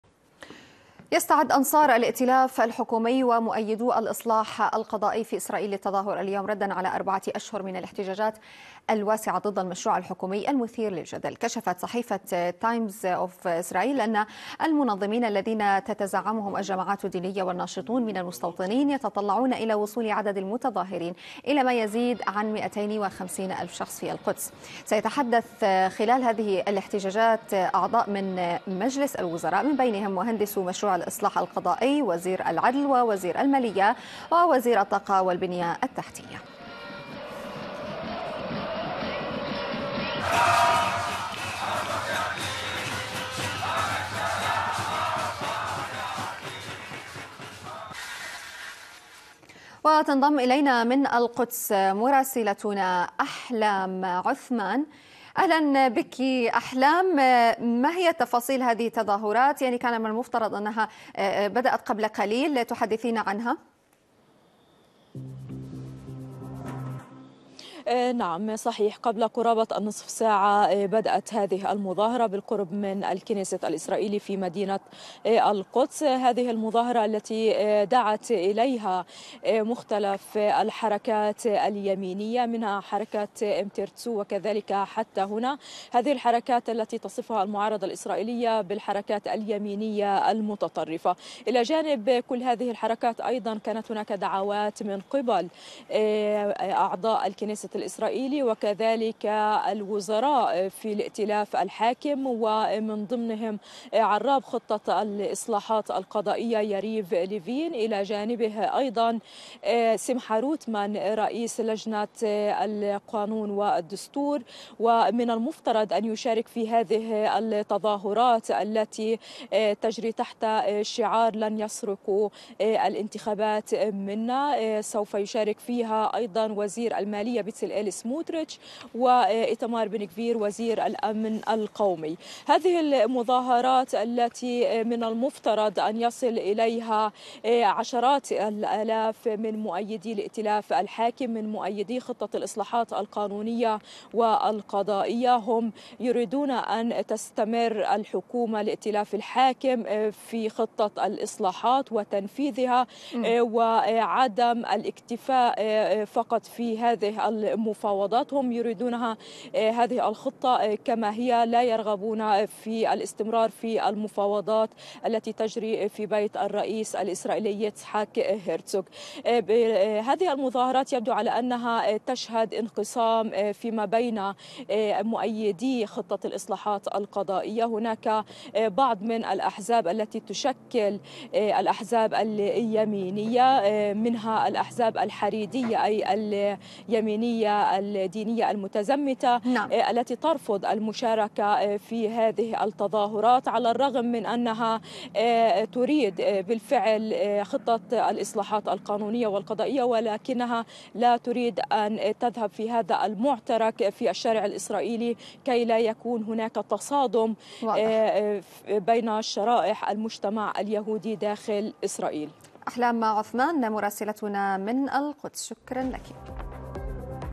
مؤيدو الاصلاحات القضائية يستعدون للتظاهر في إسرائيل
يستعد أنصار الائتلاف الحكومي ومؤيدو الإصلاح القضائي في إسرائيل للتظاهر اليوم ردا على الاحتجاجات الواسعة ضد المشروع الحكومي المثير للجدل، وكشفت صحيفة "تايمز أوف إسرائيل" أن المنظمين الذين تتزعمهم الجماعات الدينية والناشطون من المستوطنين، يتطلعون إلى وصول عدد المتظاهرين إلى ما يزيد عن 250 ألف شخص في القدس، والتفاصيل مع مراسلتنا في القدس أحلام_عثمان.

لتحميل تطبيق قناة ومنصة المشهد:
App Store